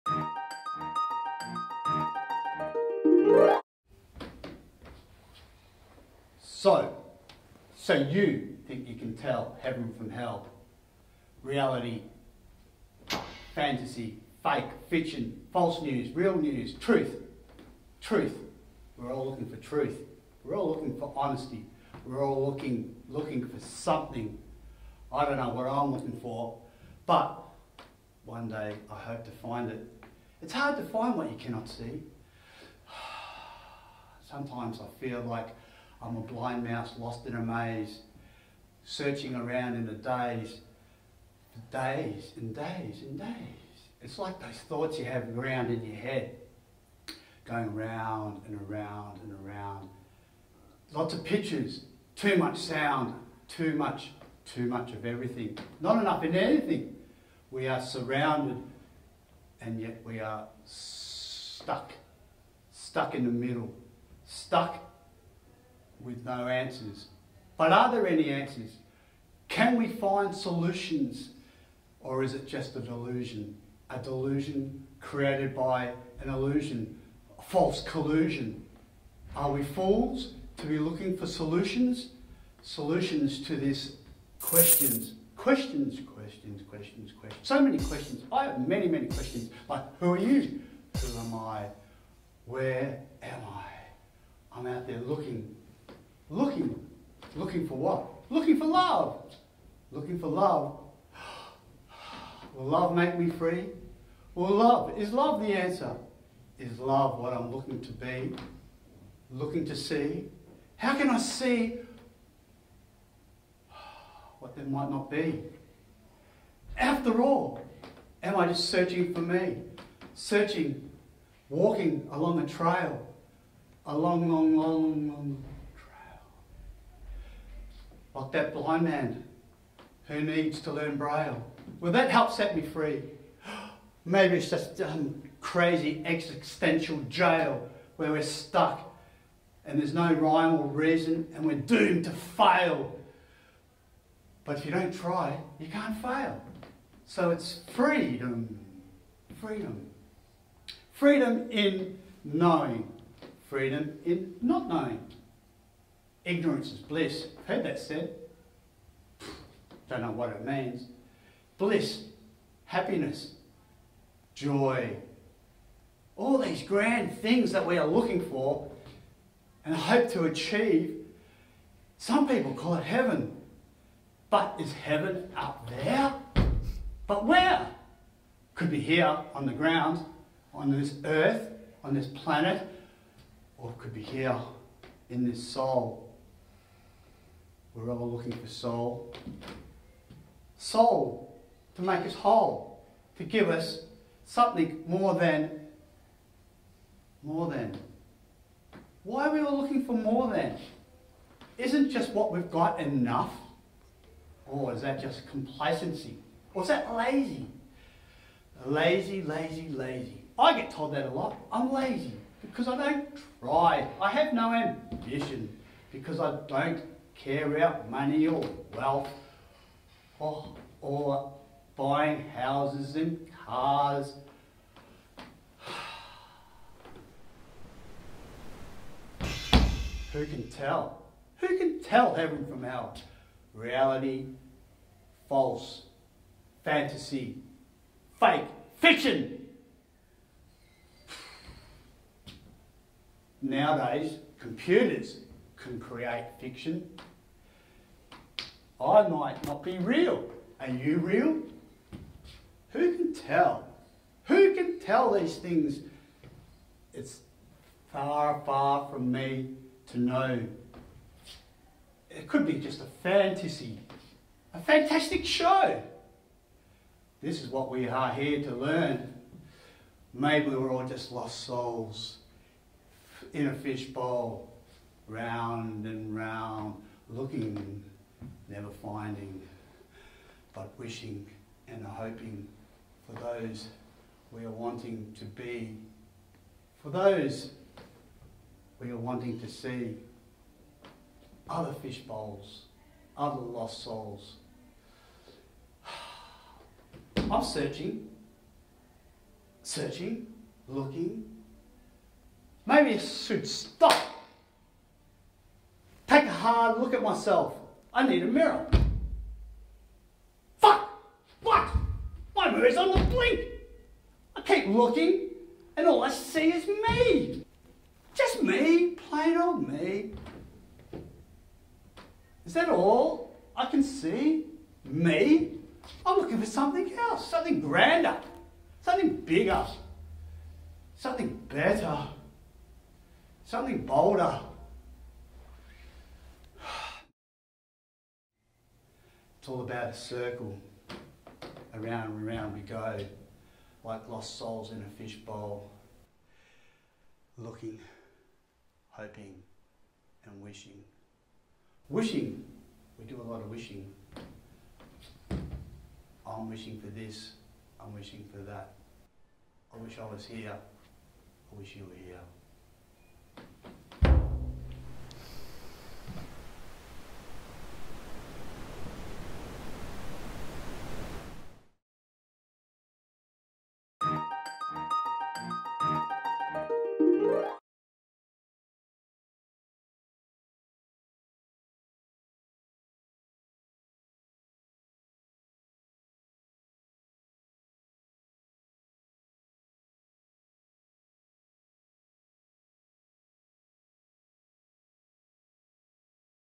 Wishing and a hoping Final webversion
Wishing & Hoping - Searching for Soul

This is the final version of this particular project, at this stage.
During the past 3 years I have been trying to develop an idea for a film & play.
This is the prototype or demo.
It’s a performance piece I’m still developing based on a rambling poem I wrote called “Searching for Lost Soul”.
I intend on getting some support to promote the concept and find some assistance to work on it and take it further

Thanks - Hope you Enjoy - Cheers